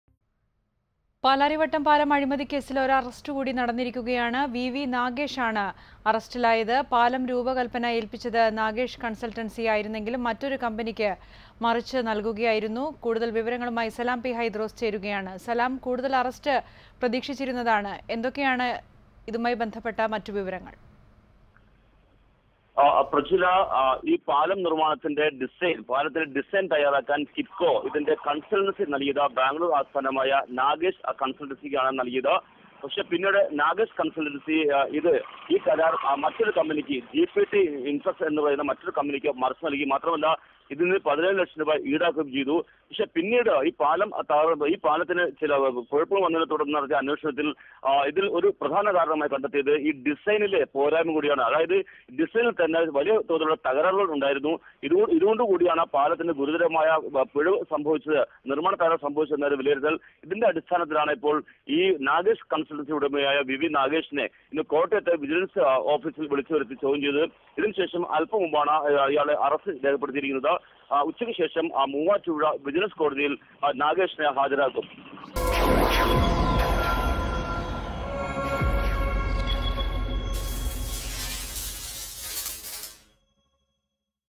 പാലാരിവട്ടം പാലം അഴിമതിക്കേസ്: നാഗേഷ് കൺസൽട്ടൻസി ഉടമ വിവി നാഗേഷ് അറസ്റ്റിൽ | VV Nagesh
VV Nagesh, owner of Nagesh Consultancy that designed the Palarivattom flyover arrested in Palarivattom bridge case